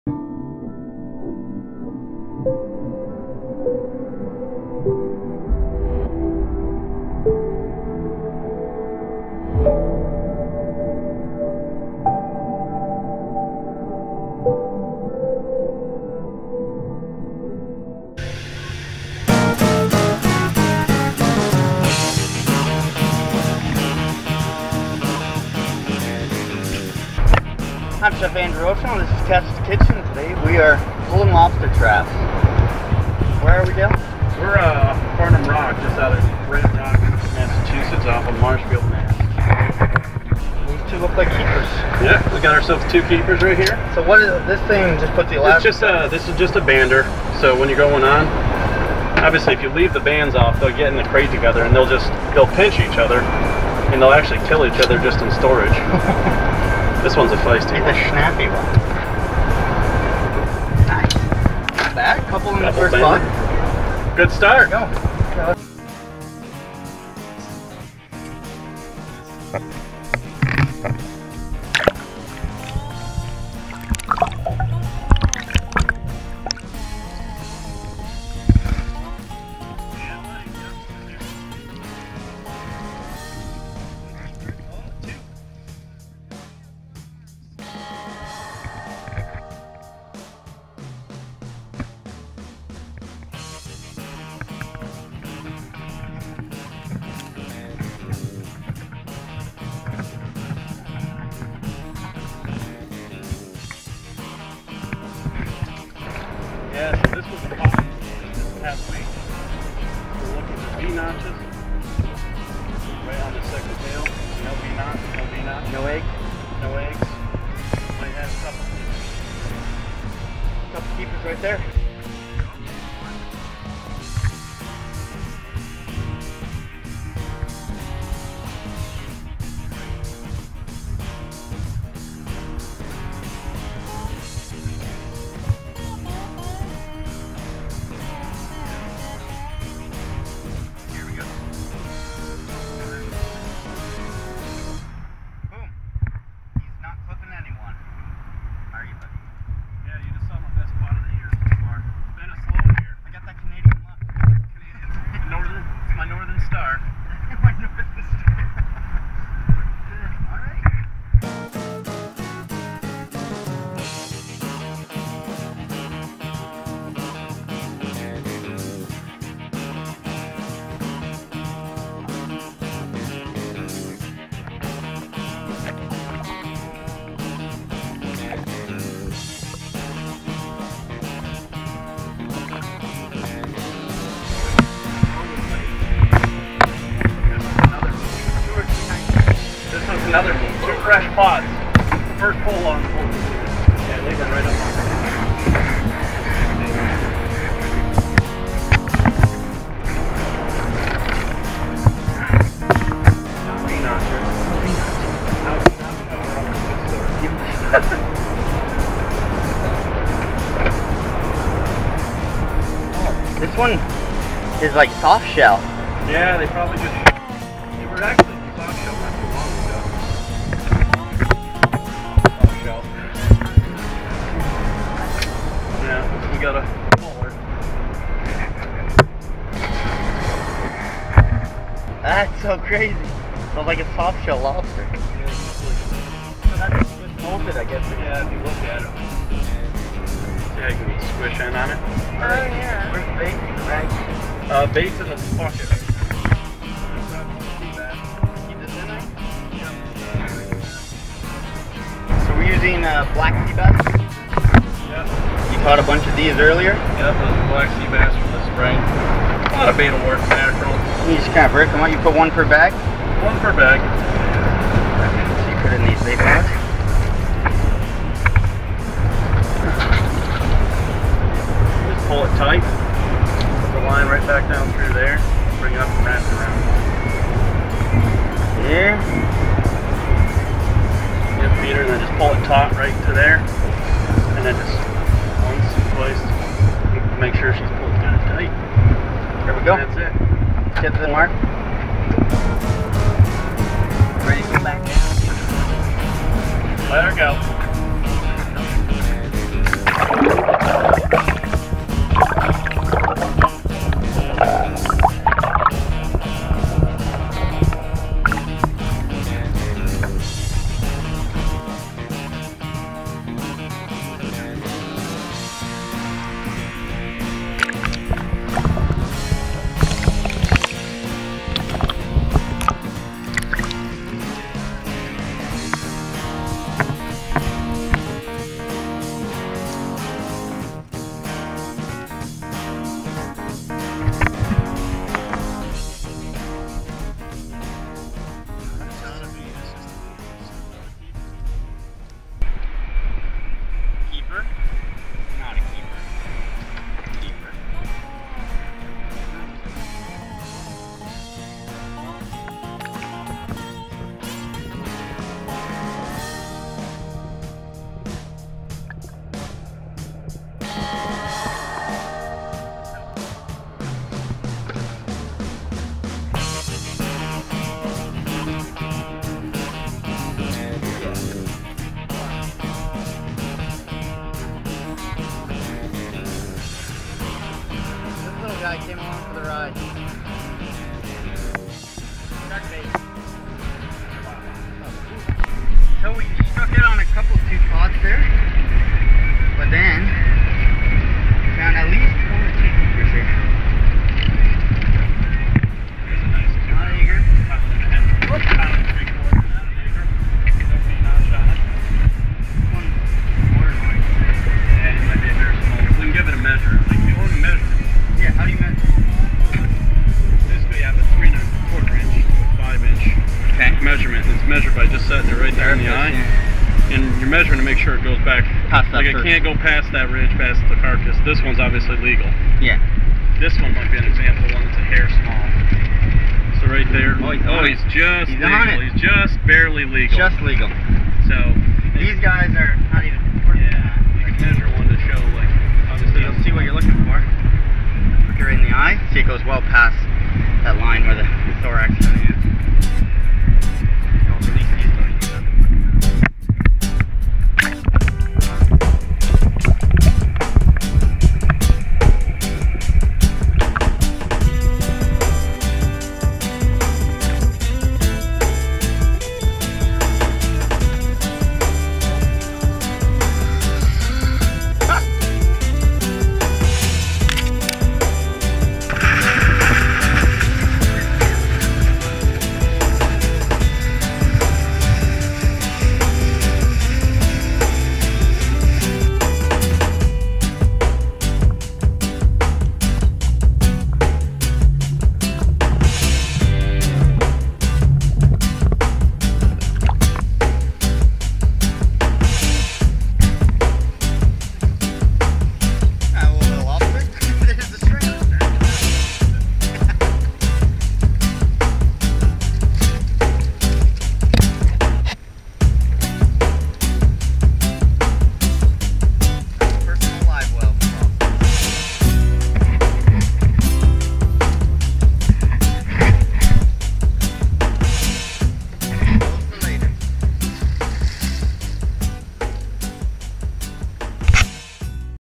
Lobster fishing in Massachusetts!
Chef Andrew pulls lobster traps near Cape Cod